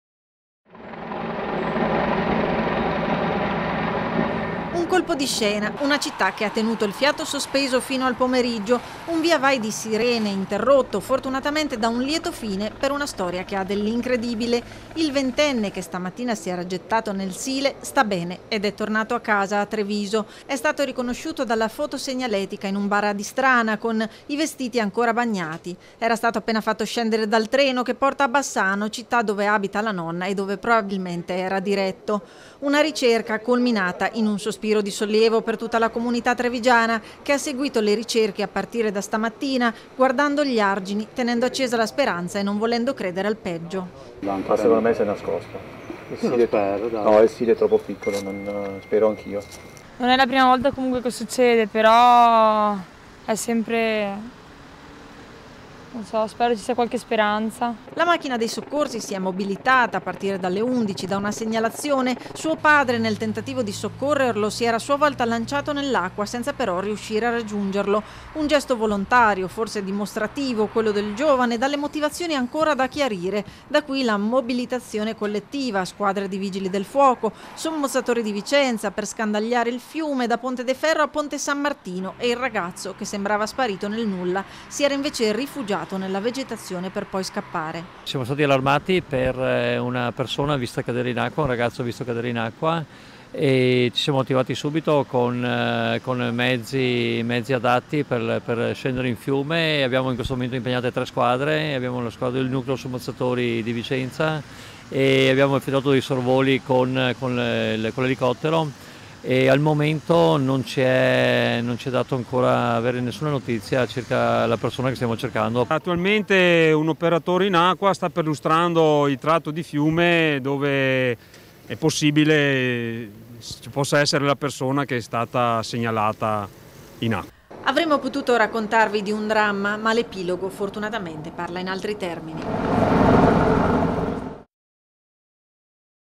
SI GETTA NEL SILE, RITROVATO VIVO DOPO ORE DI ANGOSCIA | 28/09/2020
A3 NEWS Veneto 28/09/2020 - TREVISO - Trovato vivo il ventenne che stamattina si era gettato nel Sile. Dopo essersi nascosto nella boscaglia ha preso il treno per Bassano. E' stato riconosciuto in un bar ad Istrana. Ora è a casa e sta bene - Intervistati CLAUDIO MENEGHETTI (ISPETTORE ANTINCENDIO VIGILI DEL FUOCO), LUCA SCALDAFERRO (Caporeparto Sommozzatori) - Servizio Luisa Bertini, immagini Nicola Marcato, montaggio Renato Capoia - Segui Antenna Tre anche sul digitale terrestre! Visita il sito